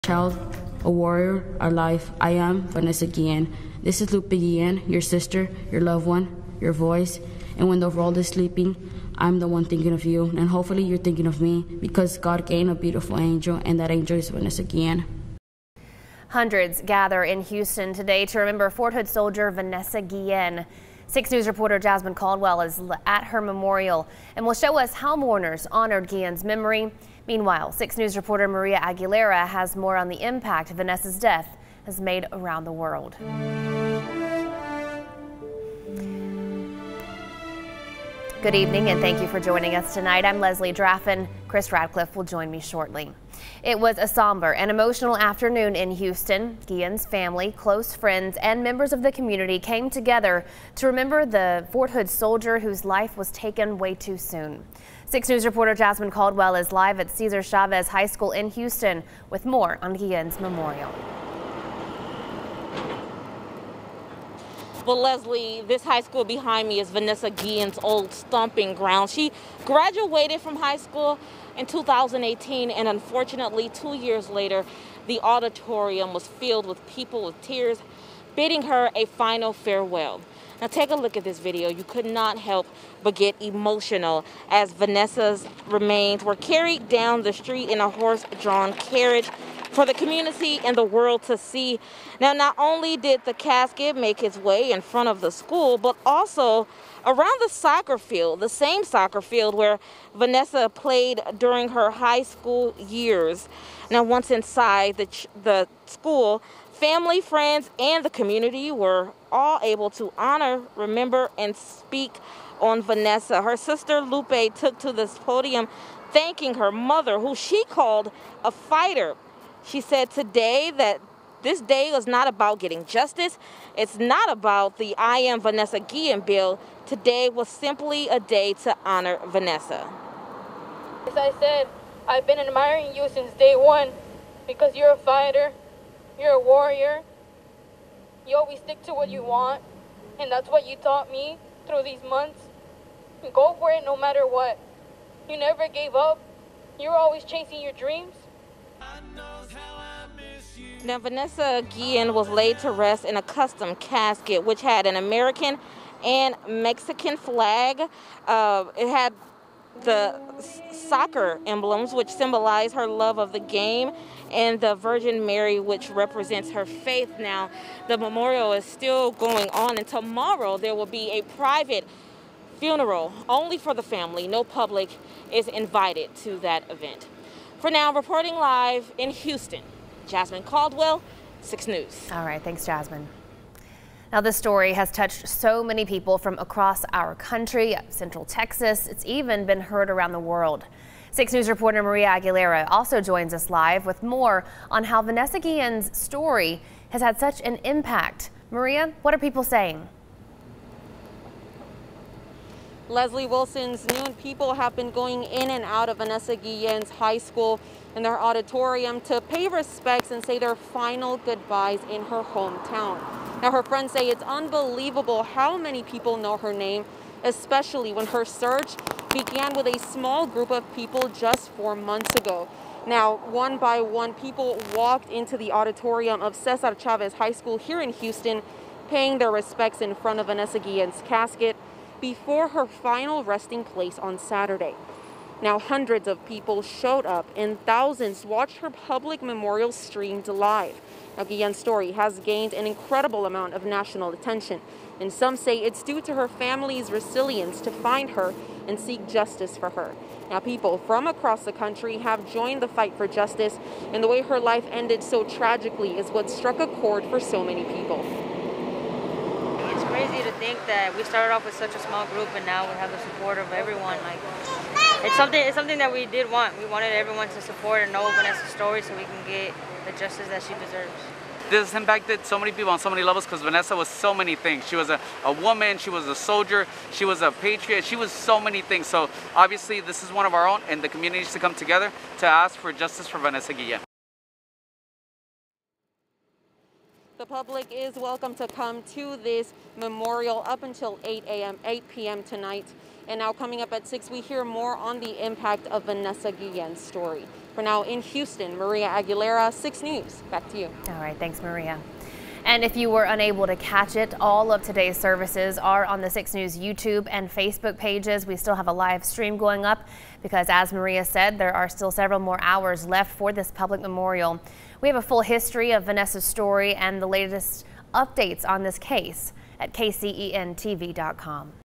Hundreds show up to pay respects at memorial for Fort Hood soldier Vanessa Guillen
Vanessa Guillen's casket was brought by horse-drawn carriage to Cesar Chavez High School in Houston. Her family, friends and members of the community came to pay their final respects in a day long public memorial.